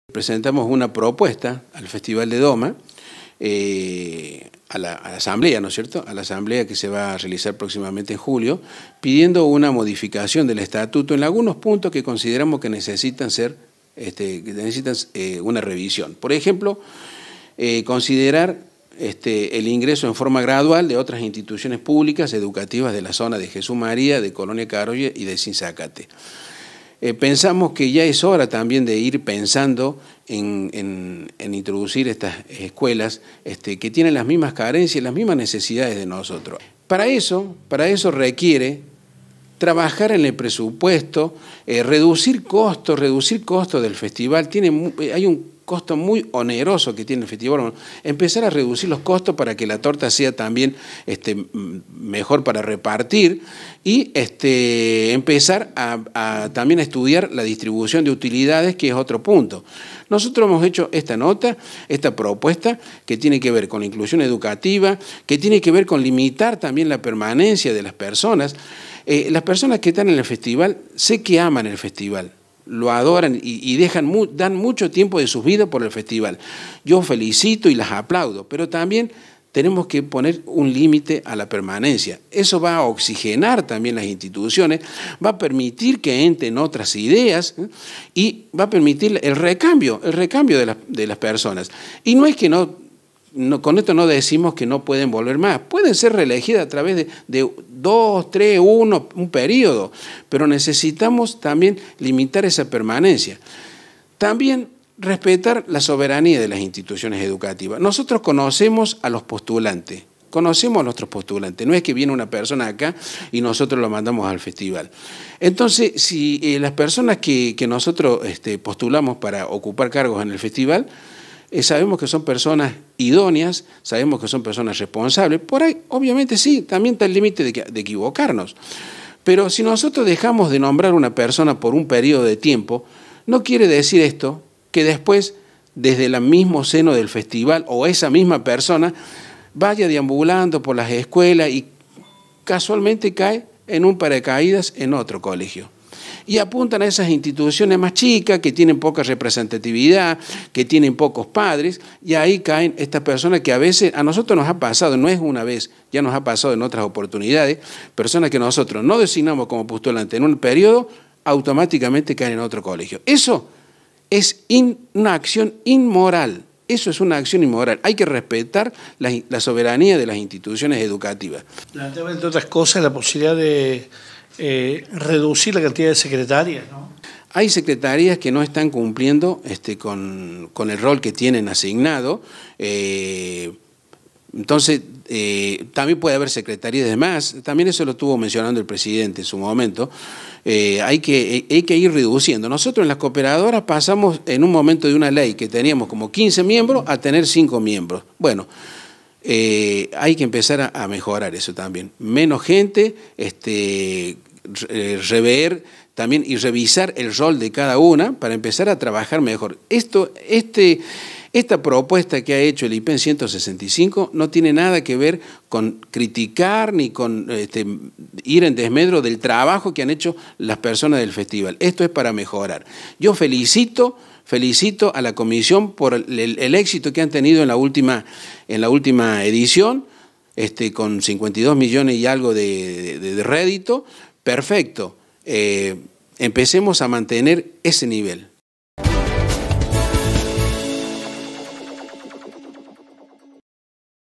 Solicitan el cambio de estatuto en l Festival de Jesús María
El colegio Bonoris de Colonia Caroya envió una carta con varios puntos para ser considerados en la orden del día de la asamblea de Cooperadoras  Escolares que integran el Festival de Doma y Folklore de Jesús María.

🎥 Prof. Salvador Lucero, director del IPEM 165 Pbro. José Bonoris.